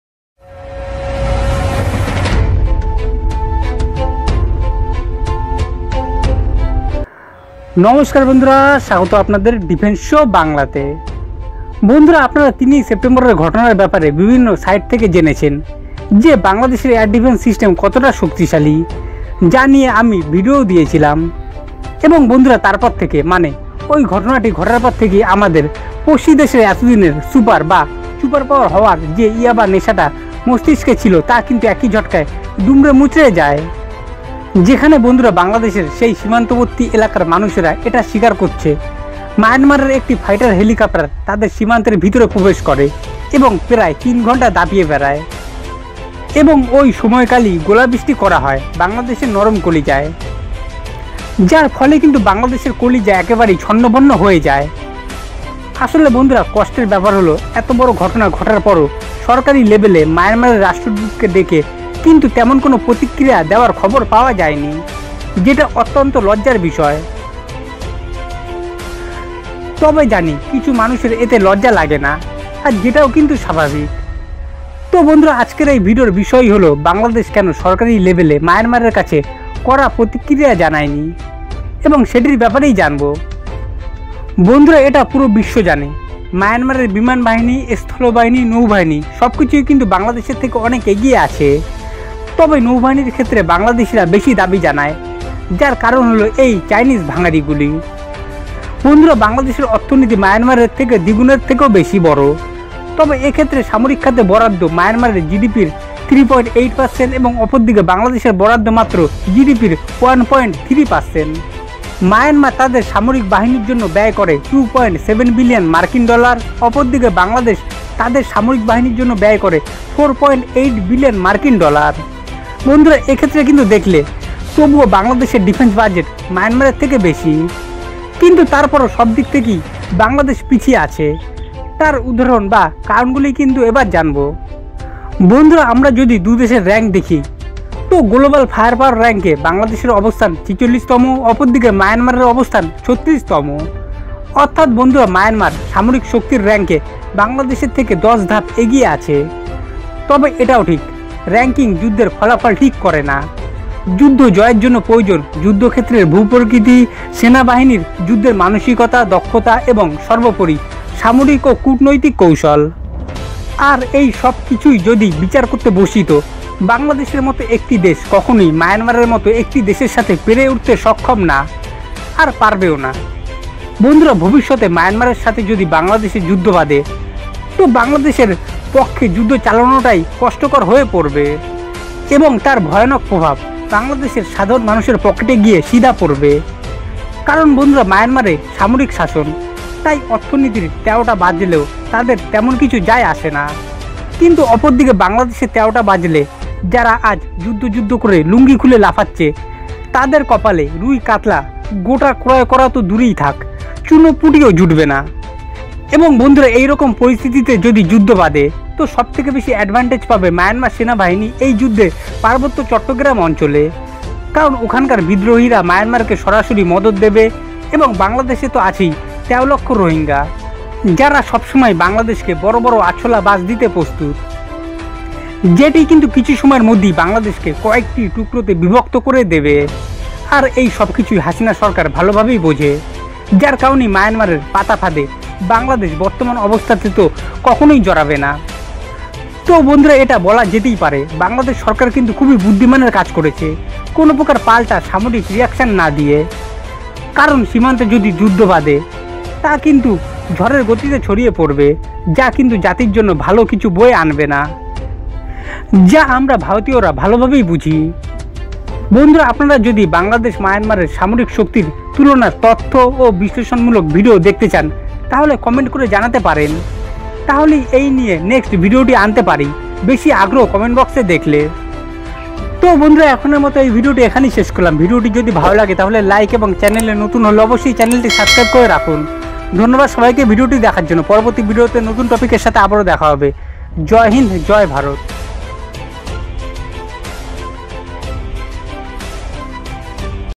কি হবে যদি দুই মহাশক্তি ময়দানে আসে? 🙏 | Bangladesh Army Vs Myanmar Army
বন্ধুরা আপনাদের সবাইকে স্বাগত ডিফেন্স শো বাংলাতে। এই চ্যানেলের মাধ্যমে ভারতের তিন বাহিনীর সামরিক বিষয়ক নিউজ ও আপডেট তুলে ধরা হবে। ধন্যবাদ।